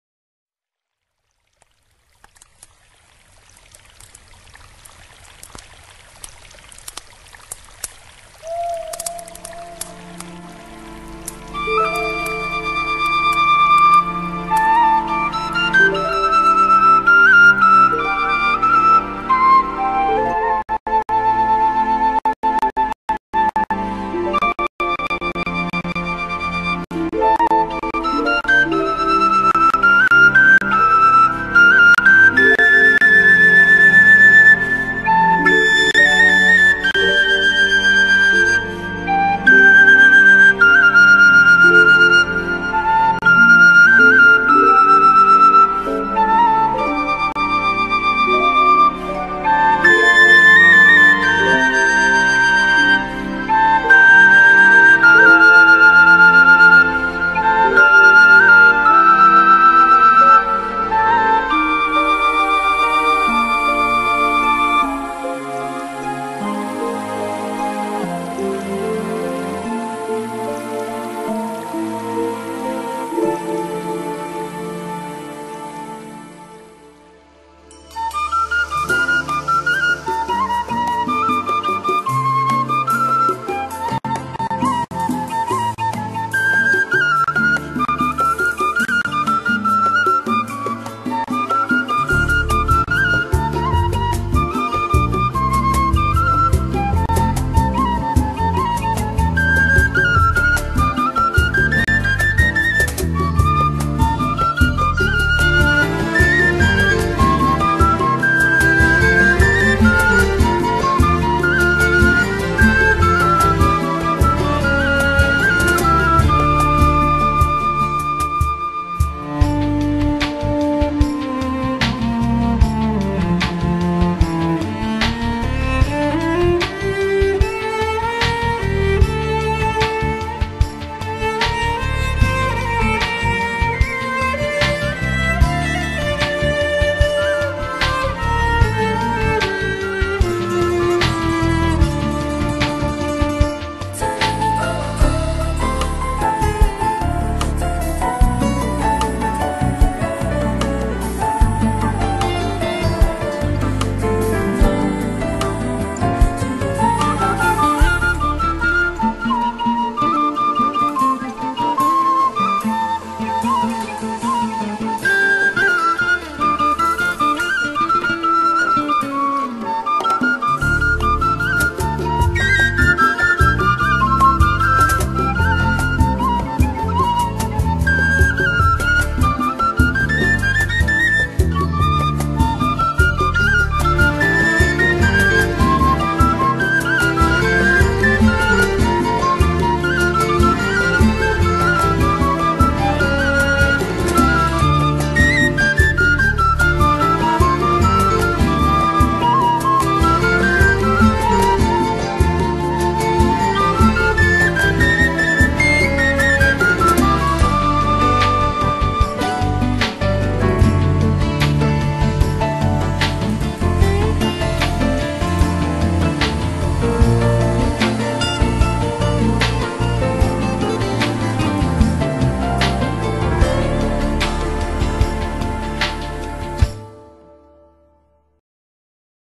Andalucía a través de los cinco sentidos. El Tacto
Celebración del día de Andalucía 2013. CEIP P. Pilar Martínez Cruz. Huelva